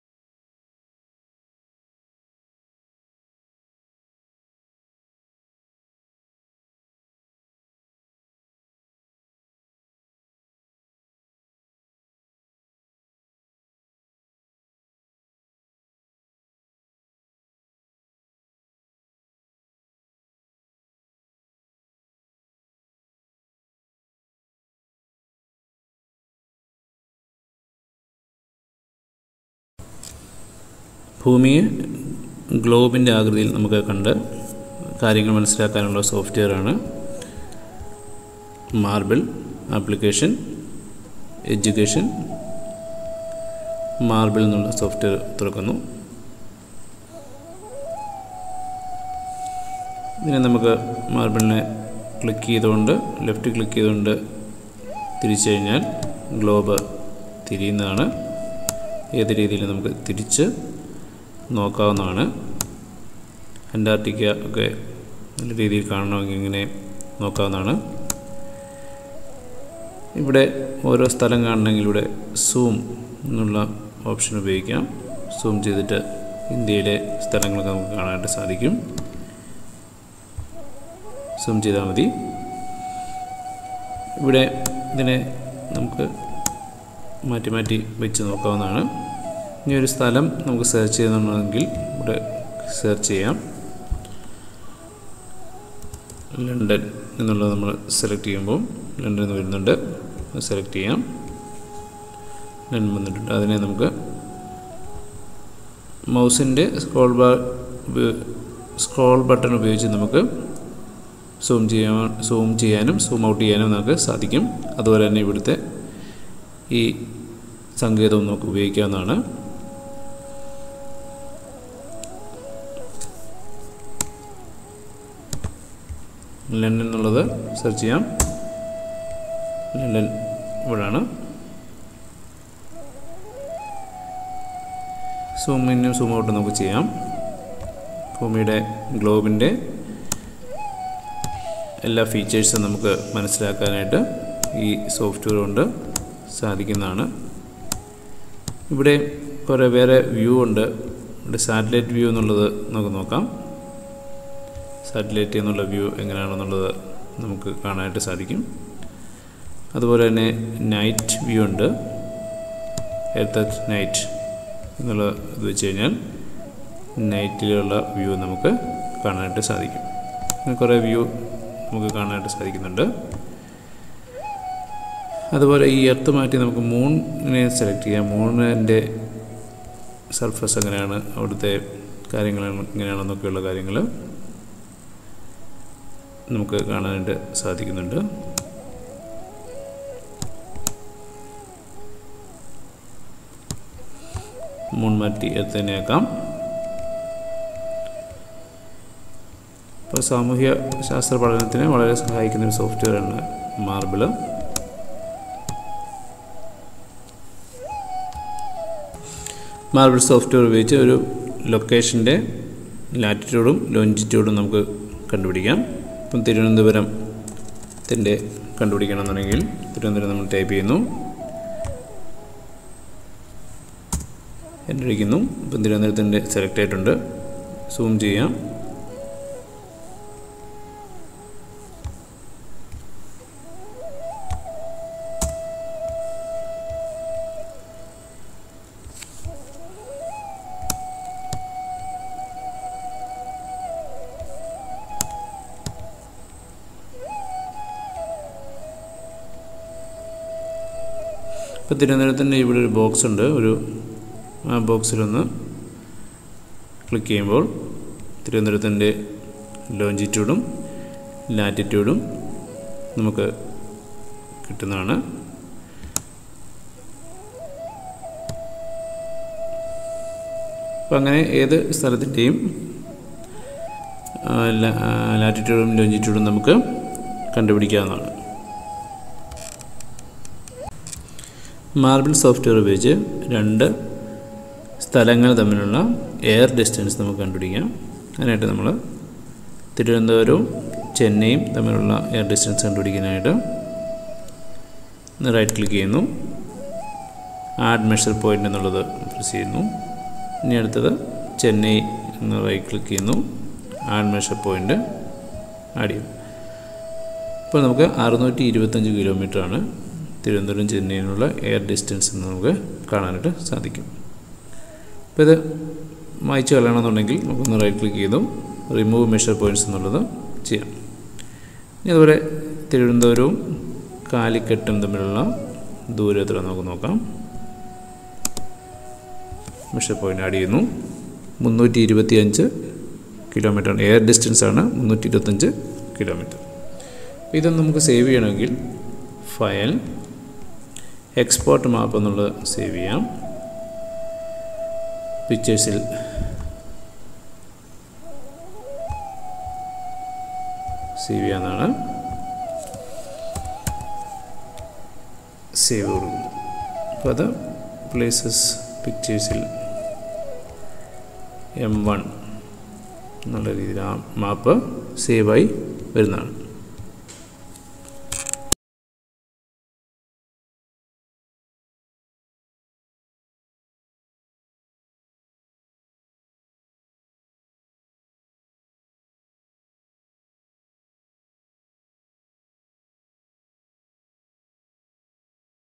MARBLE-VIRTUAL GLOBE-DIGITAL GLOBE WHICH CAN BE EASILY USED IN CLASS ROOM
MARBLE-VIRTUAL GLOBE in Ubuntu 18.04-DIGITAL GLOBE WHICH CAN BE EASILY USED IN CLASS ROOM-DIGITAL MAP TO FIND OUT DISTANCE BETWEEN PLACES-Full tutorial in Malayalam